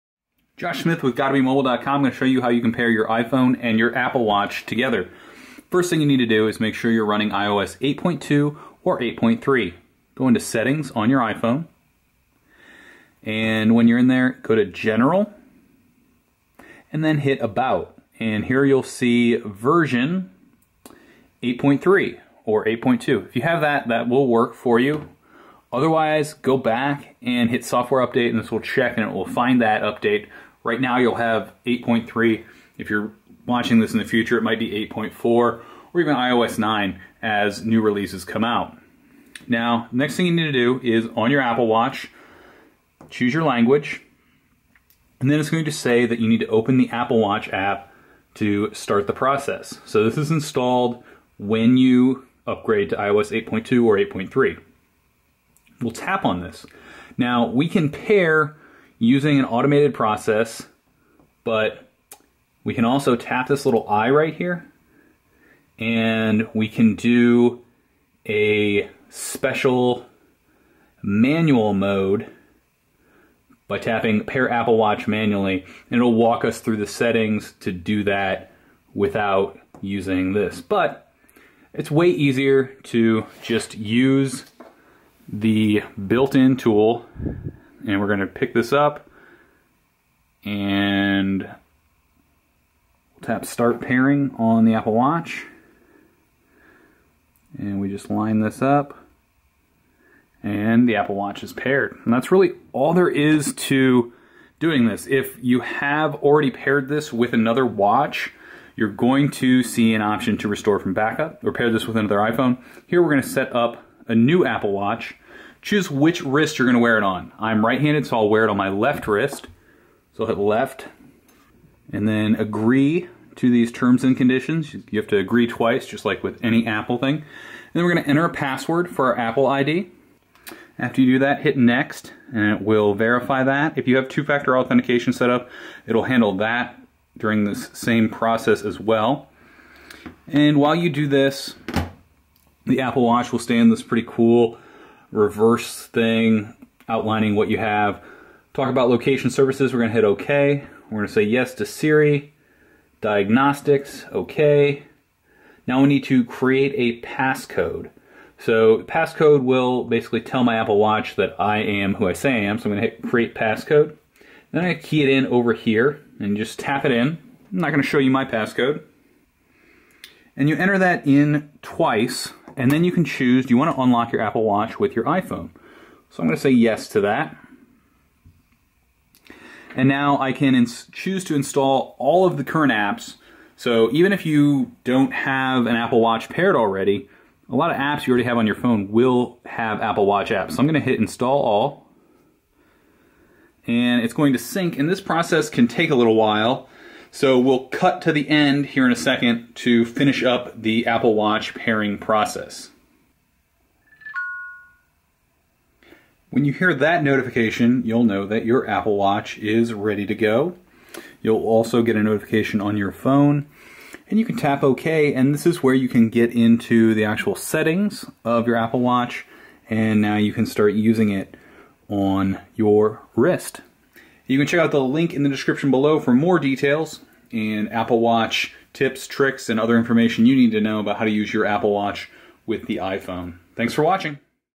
How to Pair Apple Watch with the iPhone
Learn how to pair your Apple Watch with any iPhone 5 or newer. The process takes about 10 to 15 minutes depending how many Apple Watch apps you need to sync to your new Apple Watch. You can also use this guide if you're going to pair your Apple Watch to a new iPhone or restore your Apple Watch from the backup. More